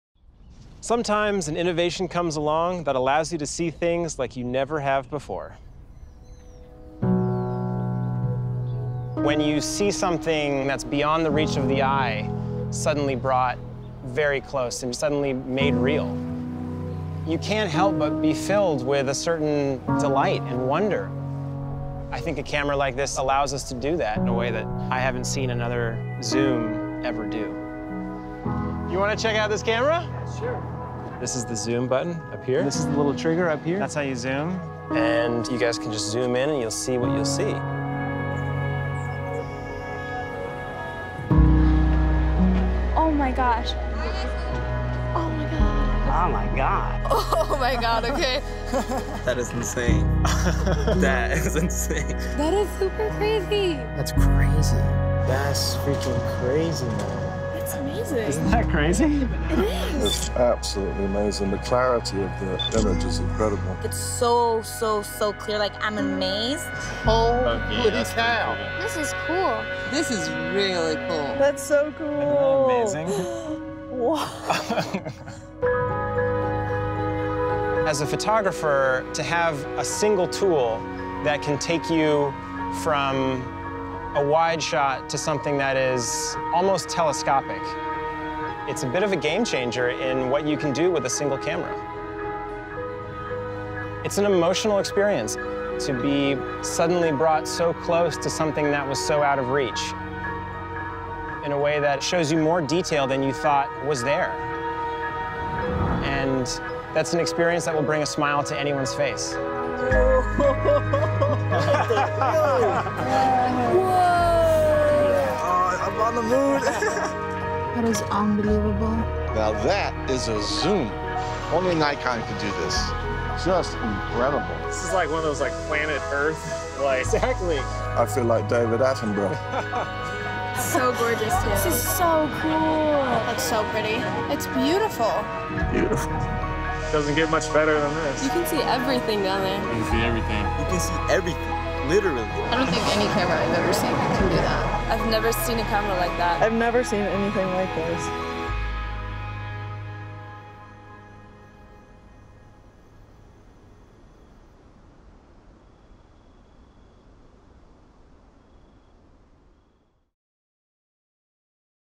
Nikon COOLPIX P1000: See Further
With the remarkable power of the 125x Zoom-NIKKOR ED glass lens, Alex Gorosh used the COOLPIX P1000 to see further. Watch as he zooms through Los Angeles.